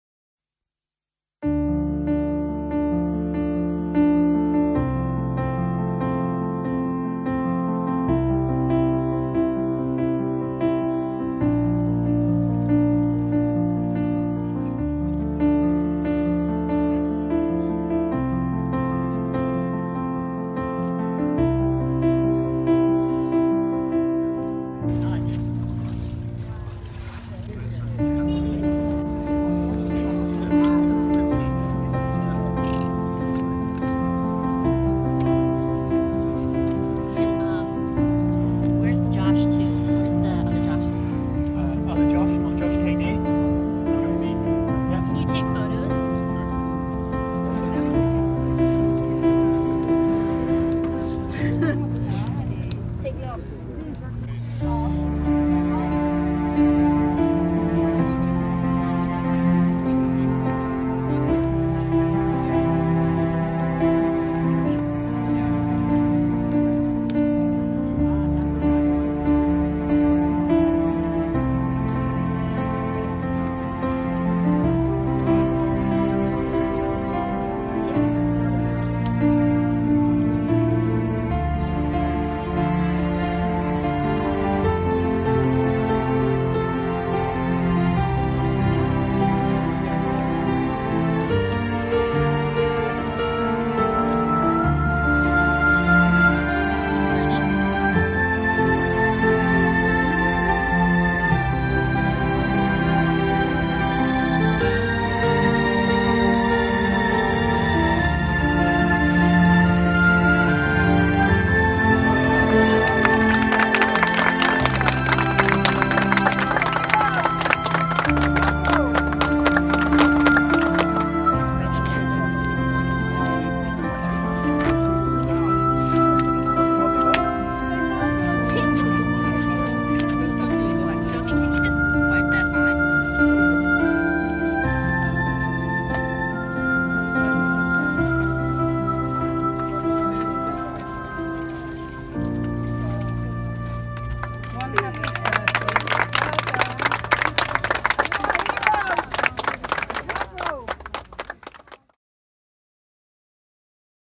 Seaturtle gets rescued after swallowing fishing net in Greece
Members of the Wildlife Sense team rescued a Caretta Caretta turtle in the harbor of Argostoli, Kefalonia. This turtle got helped, but fishing nets are number 1 death cause of seaturtles. Keep the oceans clean!